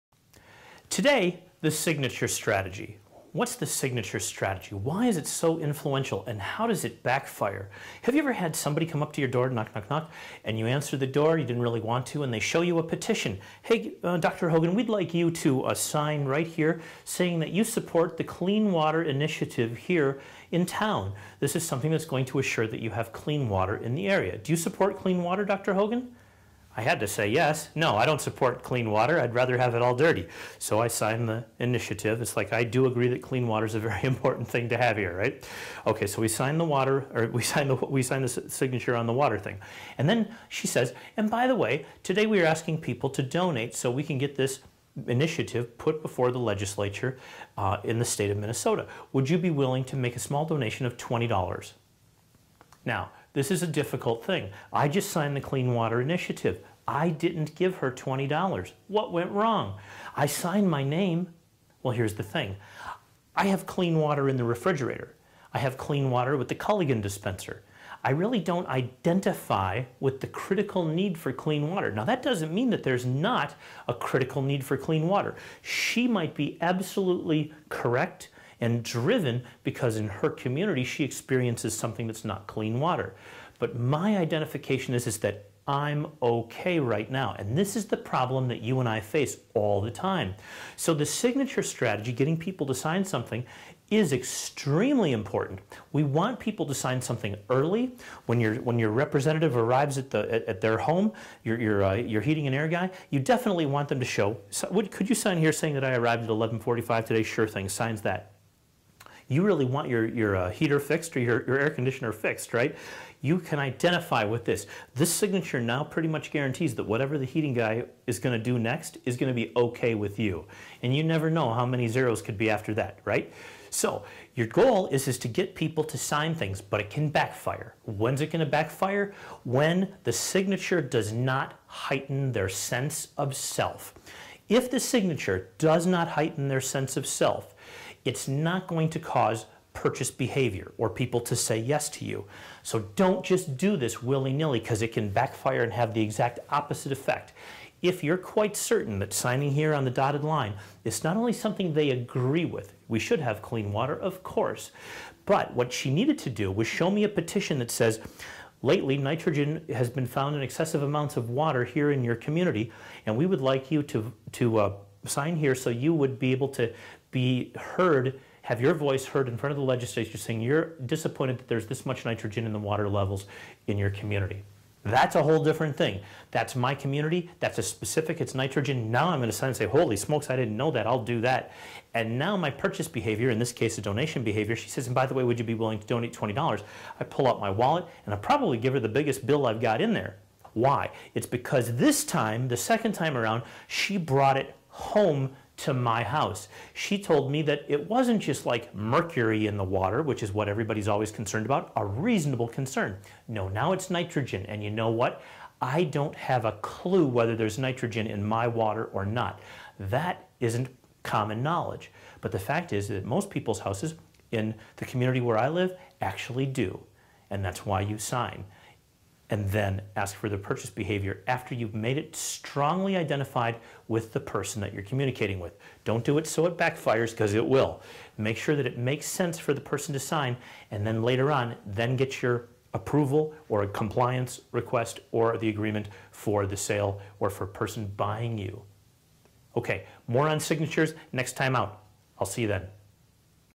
Persuasion Technique - Get their Signature? Kevin Hogan
"Sign here please." There are plenty of reasons to cause someone to sign a paper to increase compliance. But watch out! The technique can EASILY backfire and cost you dearly. This series of videos on one of our most impacting identification traits, our signature, changes how we decide and what we buy. To hire Kevin Hogan for your next event or meeting